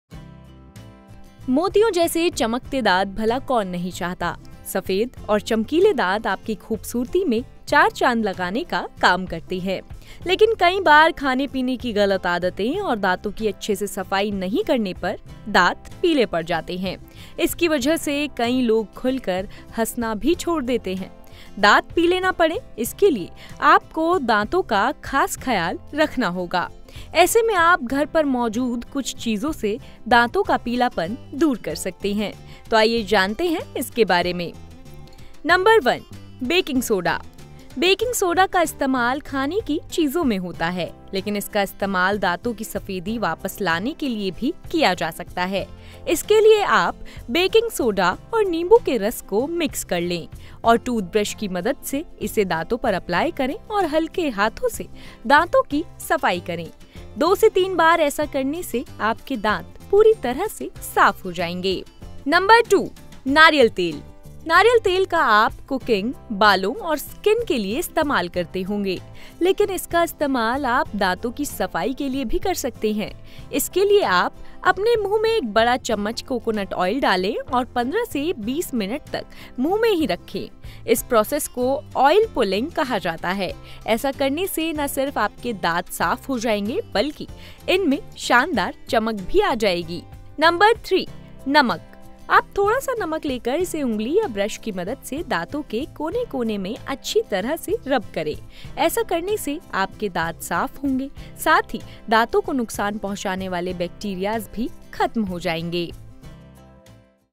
Teeth Whitening: दांतों का पीलापन होगा दूर, इन 3 घरेलू उपाय से पाएं मोतियों जैसे चमकते दांत !
Teeth Whitening: कई बार खाने-पीने की गलत आदतें और दांतों की अच्छे से सफाई नहीं करने पर दांत पीले पड़ जाते हैं। दांत पीले ना पड़े इसके लिए आपको दांतों का खास ख्याल रखना होगा। ऐसे में आप घर पर मौजूद कुछ चीजों से दांतों का पीलापन दूर कर सकते हैं। तो आइए जानते हैं  इनके बारे में।